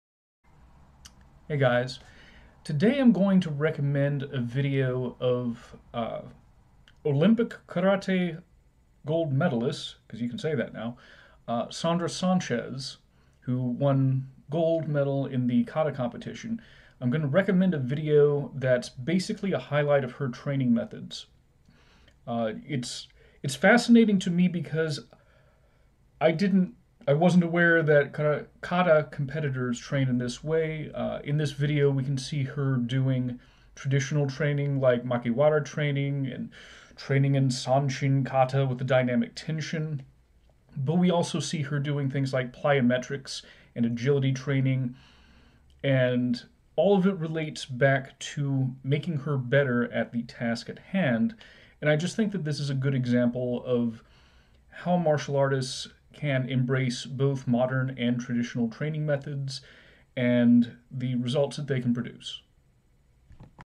Recommended Video: Olympic Gold Medalist Sandra Sanchez training highlights
Training highlights for the Olympic Gold Medalist in Women's Karate Kata, Sandra Sanchez, displaying both traditional and modern training: